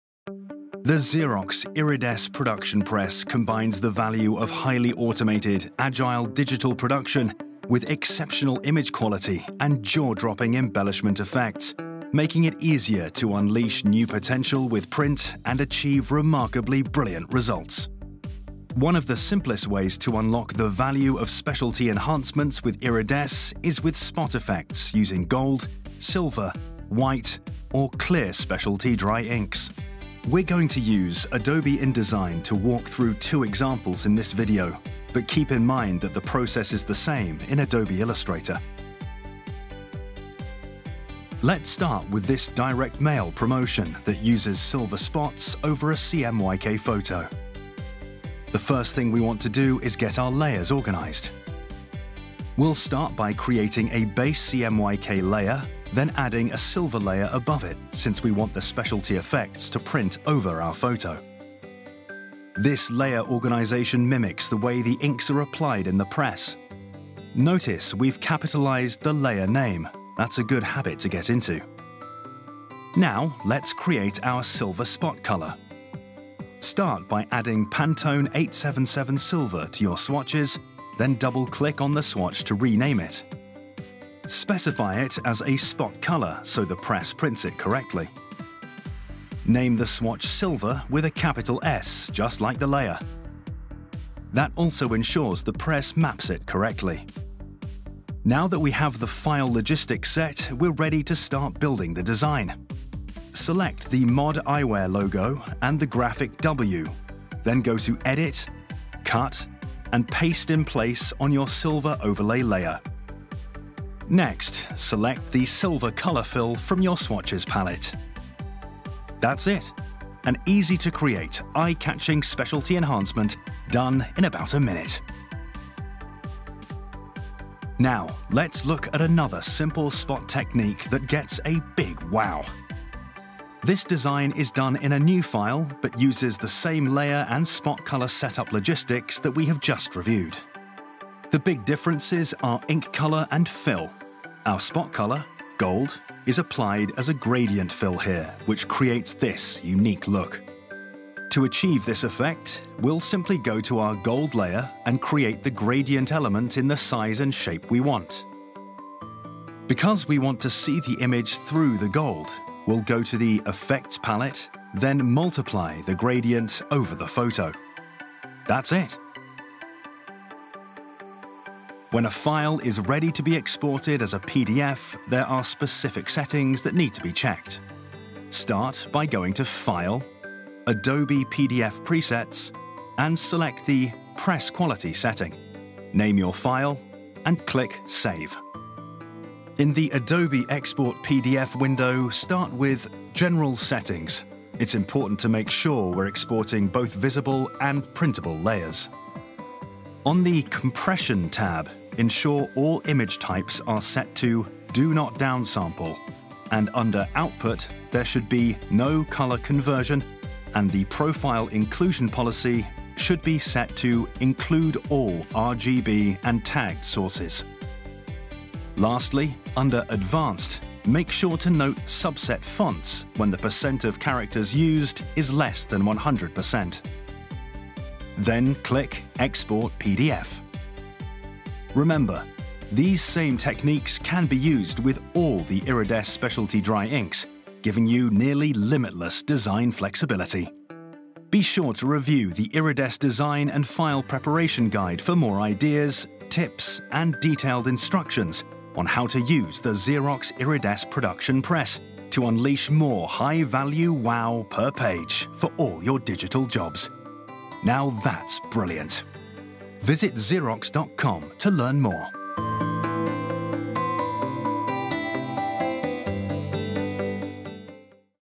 Xerox® Iridesse® Production Press: Designing for Spot Specialty Colors
*If you do not have access to the PANTONE® swatch library, you may choose any on-screen color you’d like to represent the Specialty Dry Ink as long as it is named correctly and specified as a spot color.

This video shows how to design for spot specialty colors using Adobe® InDesign® on Xerox® Iridesse® Production Press Family products.

BR26930